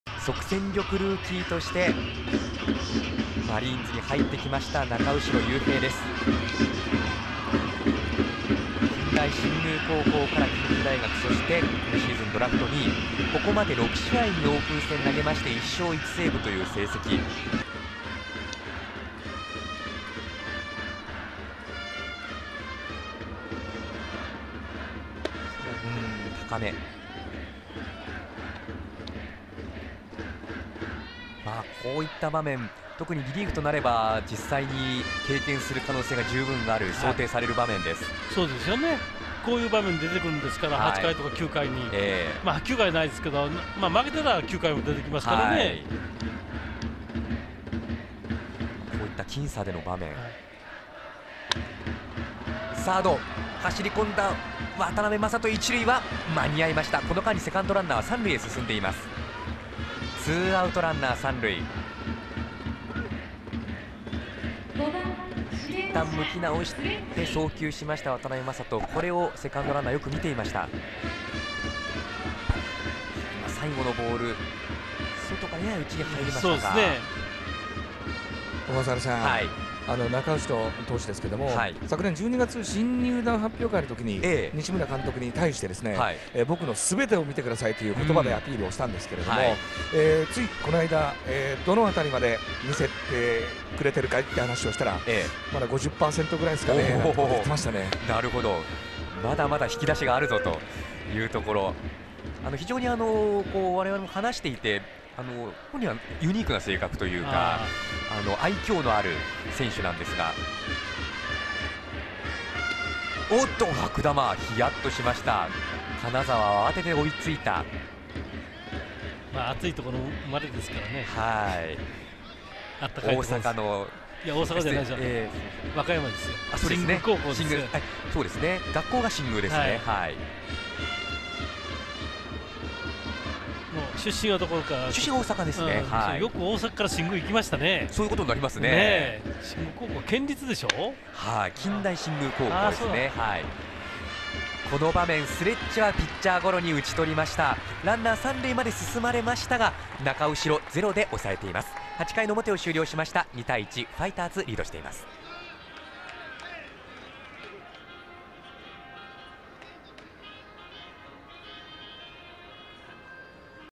これは期待できる!? ルーキー・中後が好投を披露
オープン戦で好投を続けるルーキー中後。今日も1イニングを無失点に抑えた。この変則左腕に、相手チームは手を焼くはずだ。(2012年3月24日 千葉ロッテマリーンズ対北海道日本ハムファイターズ)